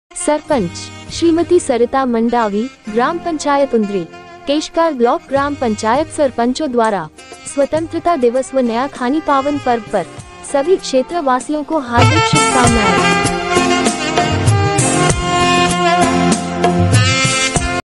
सरपंच सरिता मण्डावी , ग्राम पंचायत उन्दरी केशकाल ब्लॉक ग्राम पंचायत सरपंचो द्वारा स्वतंत्रता दिवस
श्रीमती सरिता मण्डावी
ग्राम पंचायत उन्दरी
केशकाल, कोंडागांव

सरपंच

श्रीमती सरिता मण्डावी , ग्राम पंचायत उन्दरी

केशकाल ब्लॉक ग्राम पंचायत सरपंचो द्वारा

 स्वतंत्रता दिवस व नया खानी पावन पर्व पर

सभी क्षेत्र वासियों को हार्दिक शुभकामनाऐ